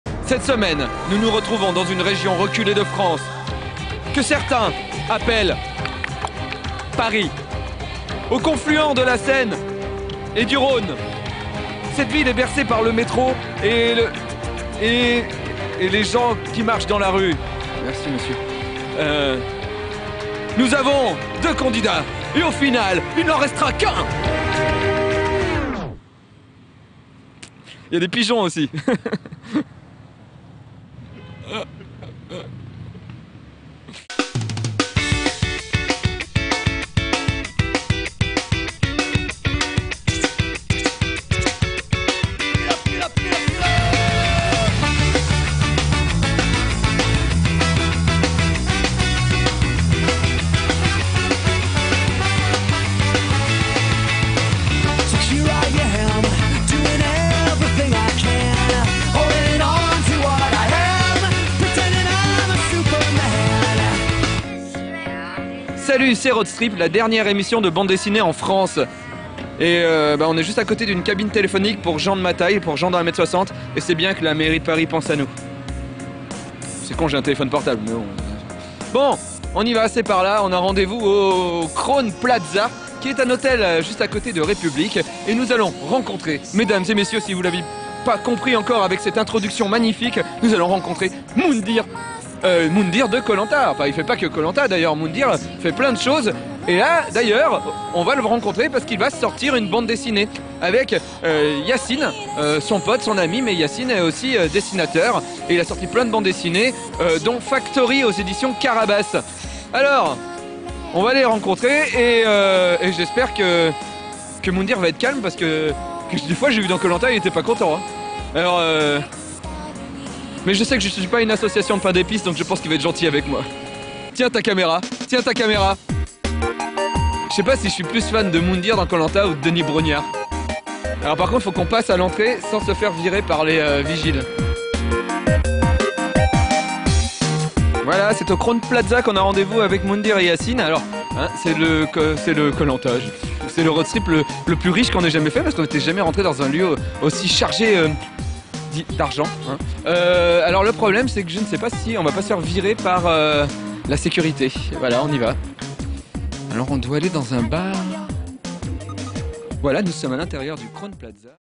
Yacine Elghorri BD Moundir is Back-Teaser
reportage sur la bd "Moundir is Back" par Yacine Elghorri et Moundir